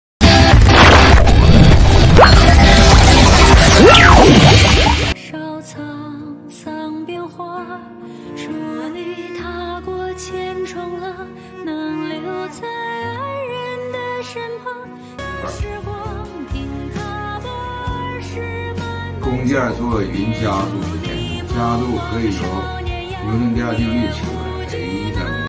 物理老师用木头自制手机支架变身“手艺人” 手写习题上网课
近日，吉林长春，特殊时期上网课，每位老师都在为网课拼尽全力。某校的一位物理老师变身“手艺人”，用木头自制手机支架，满满都是用心。